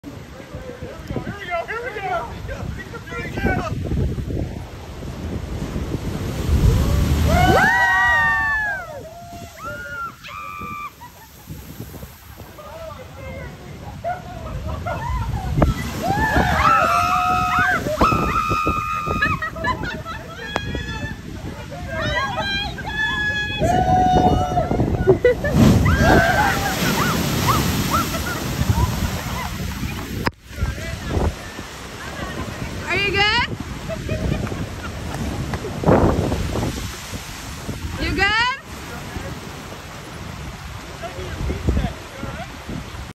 Nakalele blowhole, Maui (BE CAREFUL) we got lucky
We got to close to the waves. This was by far the biggest wave that hit the entire time we were down by the blowhole. Lesson learned. Never turn your back on the waves.